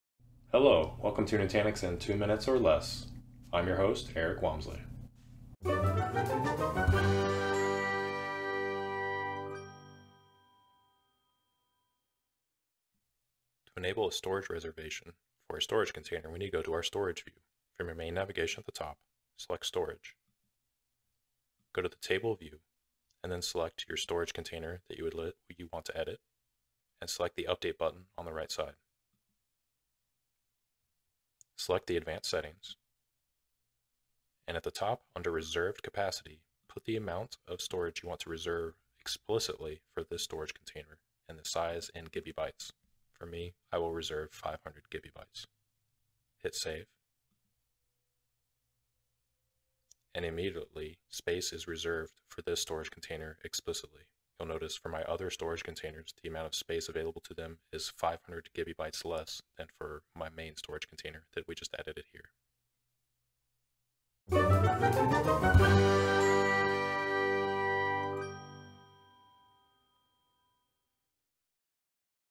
Configure a Nutanix Storage Container Space Reservation - Nutanix in 2 Minutes or Less
A space reservation on a Nutanix storage container reserves space exclusively for the selected storage container. The storage amount supplied in the space reservation can not be used by any other storage container except for the one configured. Storage reservations allows for guaranteed storage space for the selected Nutanix storage container, regardless if the storage container is using it or not.

Nutanix in 2 Minutes or Less Video Series

This video is my own, is unofficial and does not represent anyone but myself.